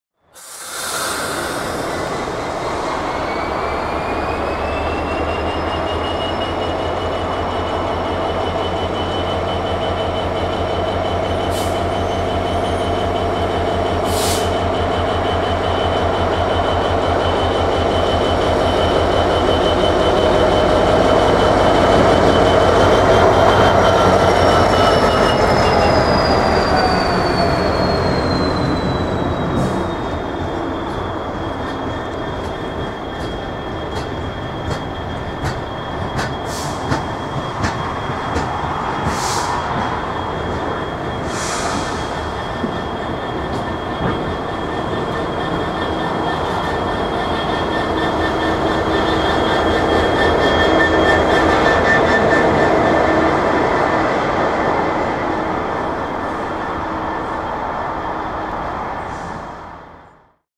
EGIP Electrification Train: 09/11/14
Direct Rail Services Class 66's 66432 TnT 66305 are seen leaving the entry to a possession between Cumbernauld and Greenhill Lower with the Kirow Crane/Pilling Rig (not sure of the exact name of this wagon) to carry out the start of the work of the next phase of EGIP.  This was filmed just outside Cumbernauld station where the recently installed wires end and where the next set will start eventually reaching the Edinburgh - Glasgow Line.

I have more clips of this train after a mini chase of it from my bike which I may upload later.